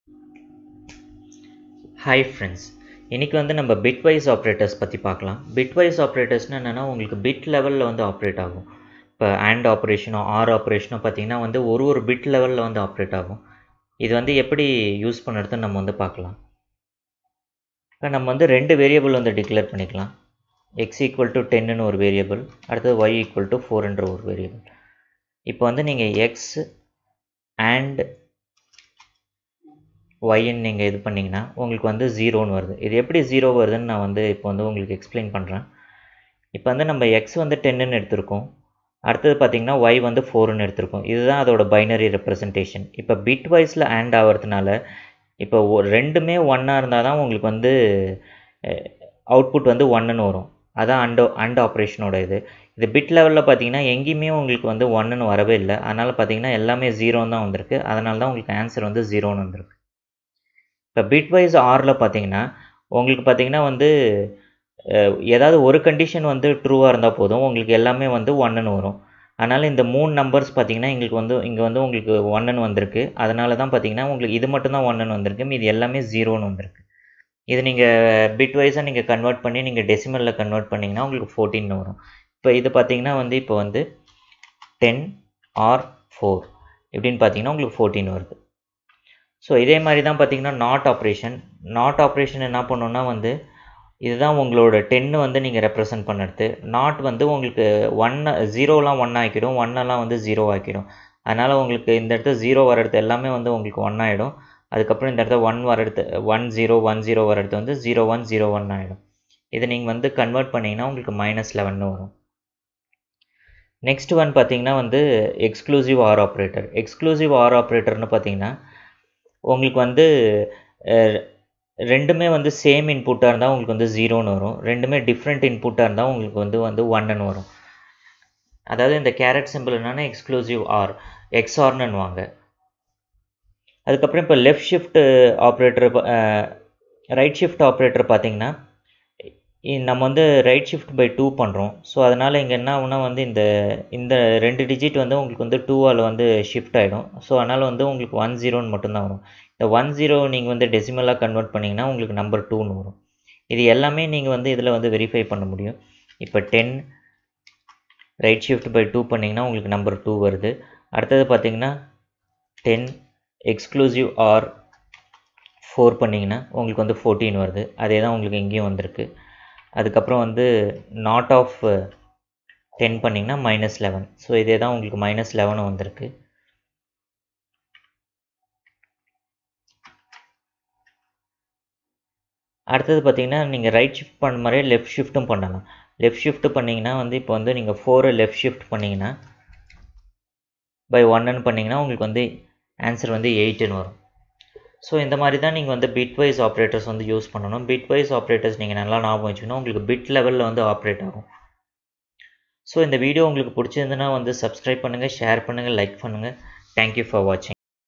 Language Tamil - Python Basics - Bitwise Operators
This is series of tutorial explaining about basics of Python for Data Science and Machine Learning foundation in Tamil